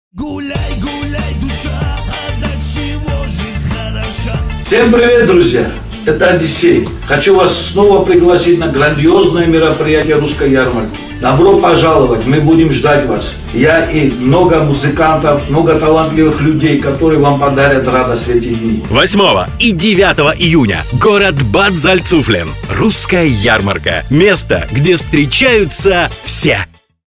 Одиссей приглашает Вас на Русскую Ярмарку 2019
Russischer Jahrmarkt: Sei dabei bei der größten Deutsch-Russischen Veranstaltung in Europa.

Wir laden Sie zu einem unvergesslichen, herausragenden
Ereignis in Deutschland mit Auftritten von vielen russischen
und deutschen Stars verschiedener Stilen und Genres ein.

Русская Ярмарка  – главное событие в русскоязычной среде Германии.

Прежде всего, это место, где можно отлично отдохнуть, узнать о новых товарах и услугах, завести полезные знакомства, отведать вкусной еды, выпить хорошего пива, ну и, конечно же, встретиться со своими земляками и одноклассниками.
Особая изюминка этого мероприятия – выступление звезд российской эстрады и местных русских коллективов.
Русская Ярмарка – место, где встречаются все!